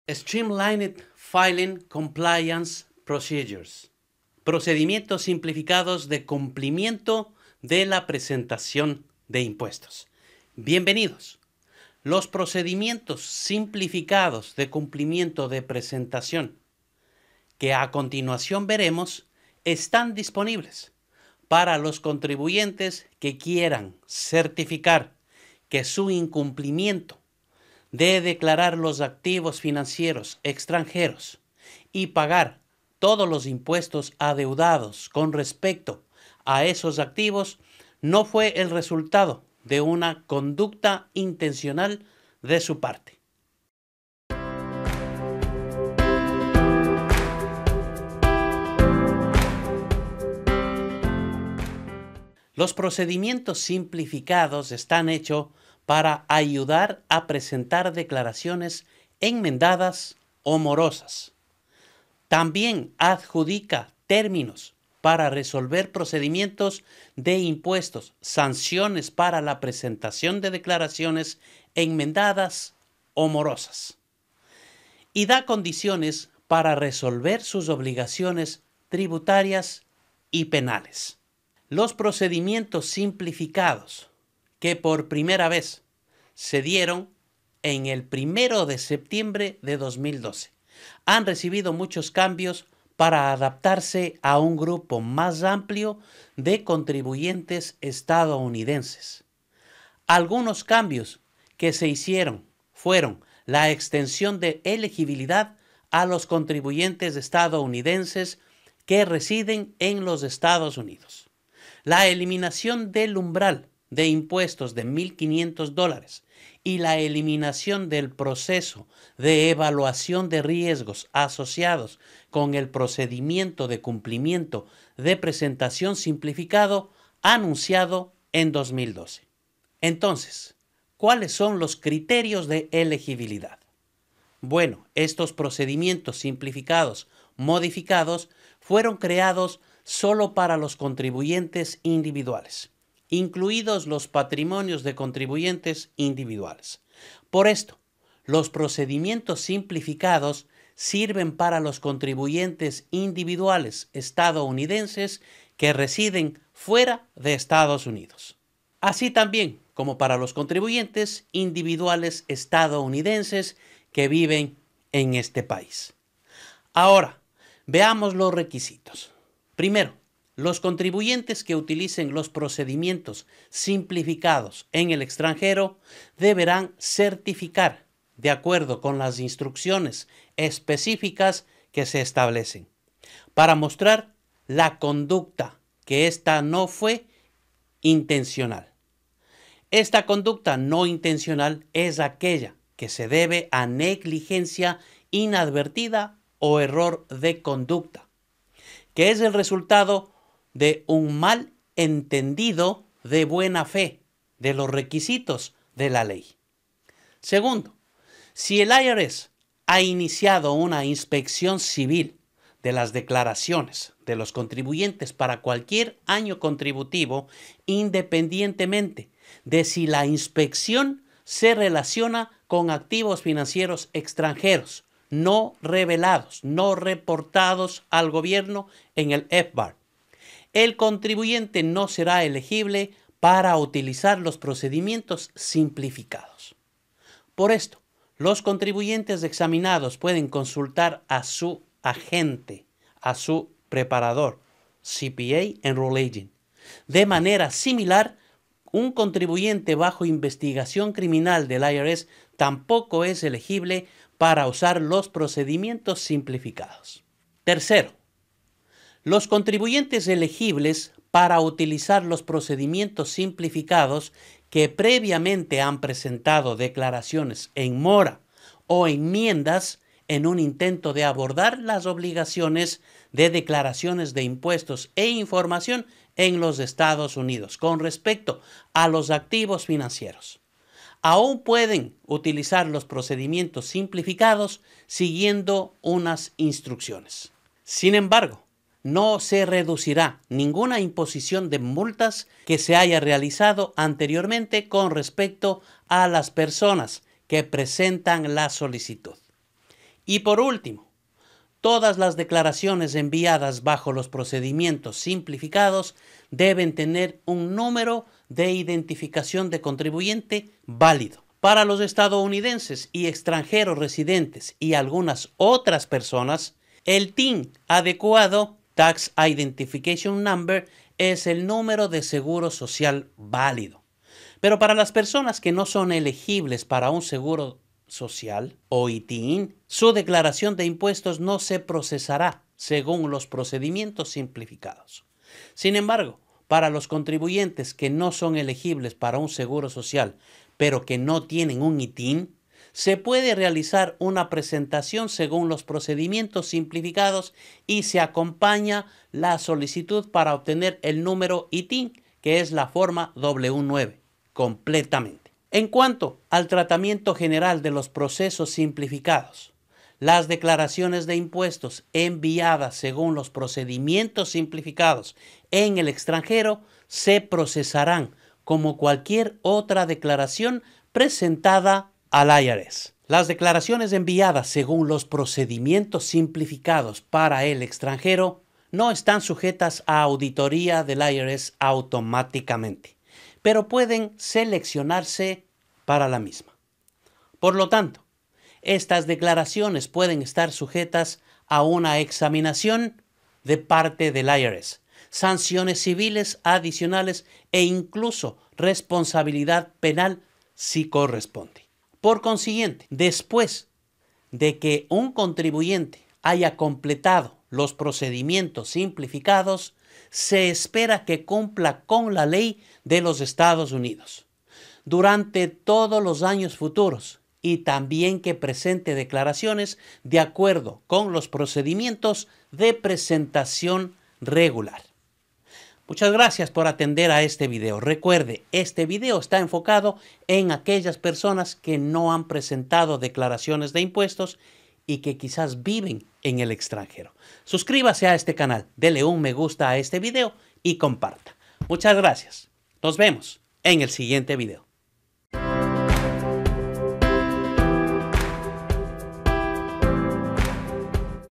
Streamlined Filing Compliance Procedures | IRS
Los procedimientos simplificados de cumplimiento de presentación que en este video veremos, están disponibles para los contribuyentes que quieran certificar, que su incumplimiento de declarar los activos financieros extranjeros y pagar todos los impuestos adeudados con respecto a esos activos no fue el resultado de una conducta intencional de su parte. Quiere saber de qué se trata, no se lo puede perder. Bienvenidos